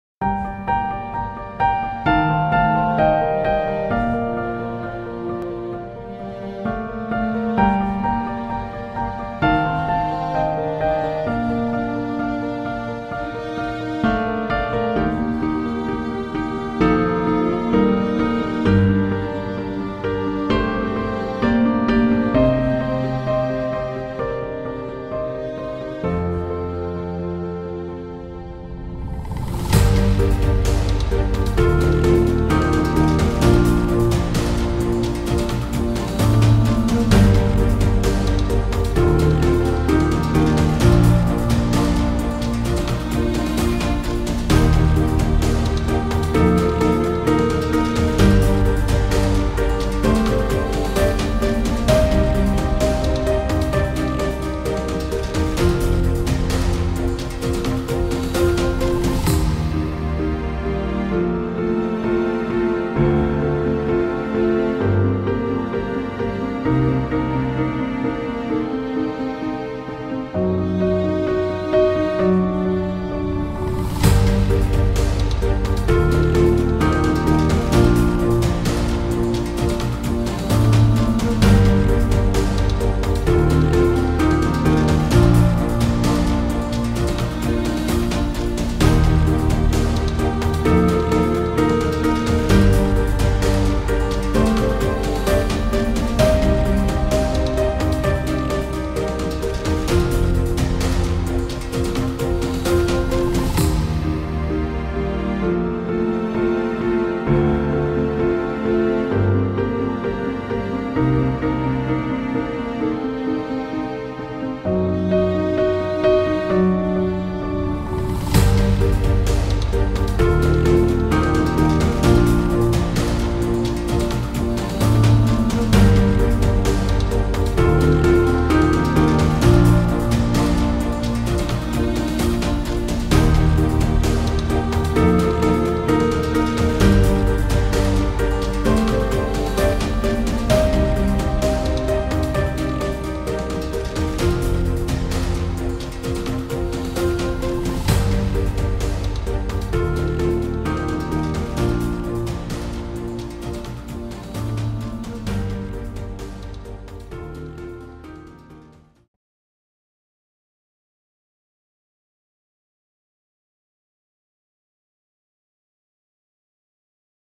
Codnor Castle October 2019
A compilation of drone footage taken for the Codnor Castle Heritage website.
Filmed using the Yuneec Typhoon H drone